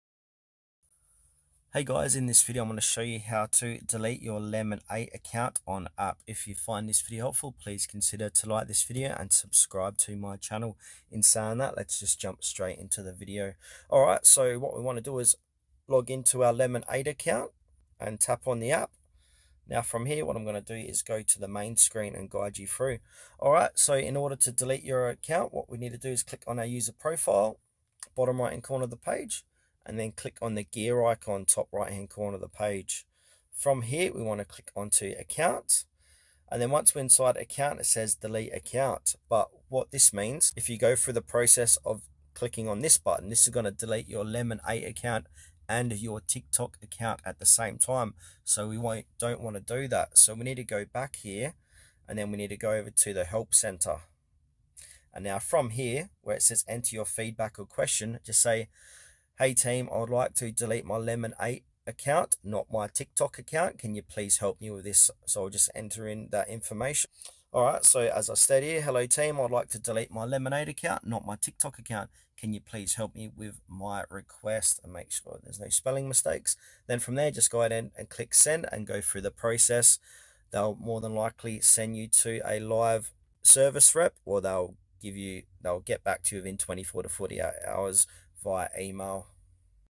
How To Delete Lemon8 Account On App
In this tutorial, we’ll show you how to delete your Lemon8 account directly from the app. If you no longer wish to use the platform, we’ll guide you step-by-step through the account deletion process. Follow along to ensure your account is removed properly.